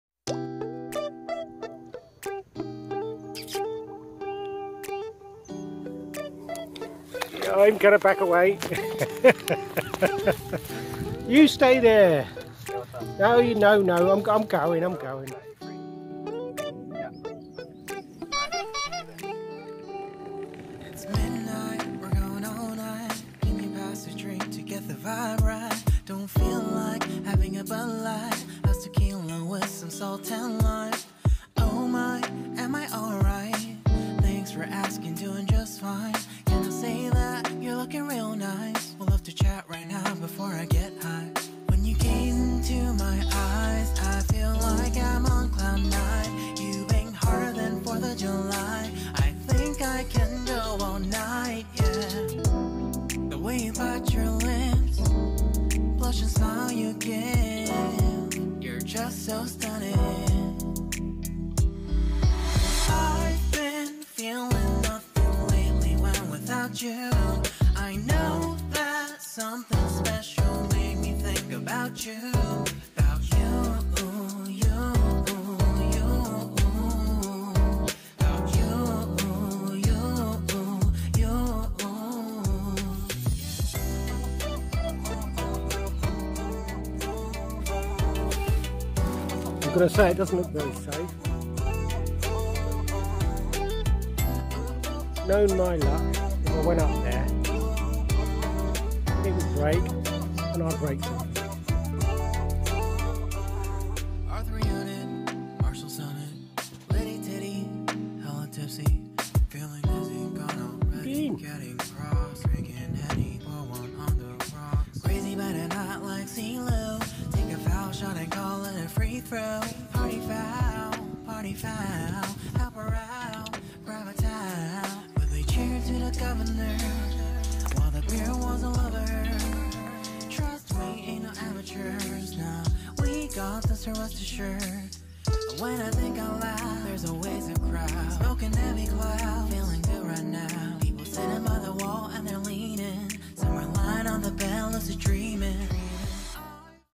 MAGICAL BIKE RIDE
stopping by to look in the local magic store... and as a bonus... seeing one of those hover bikes 😉😋 parked up.  getting told of by a swan who wanted to give me a piece of his mind 😱 ... meeting some geese who wanted me to feed them... finding a woodworm infested tree den and much more... Thanks for Watching my friends 🤗